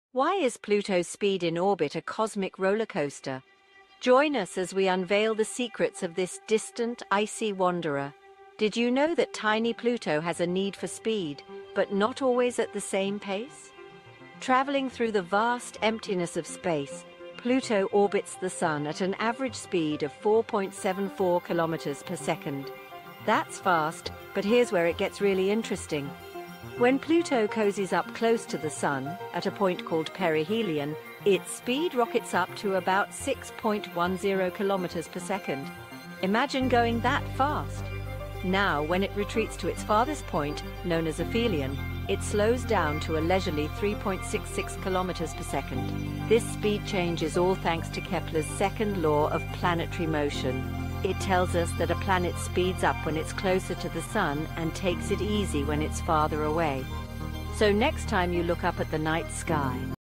Pluto's Speedy Secrets A Cosmic Dance
Discover the fascinating mysteries of Pluto's swift orbit and dynamic interactions in our solar system! Join us as we unravel the secrets behind its rapid movement, unique rotation, and intricate dance with its moon Charon. From its icy surface to its surprising atmosphere, explore how Pluto continues to captivate astronomers and space enthusiasts alike. Don't miss this cosmic journey to the edge of our solar system!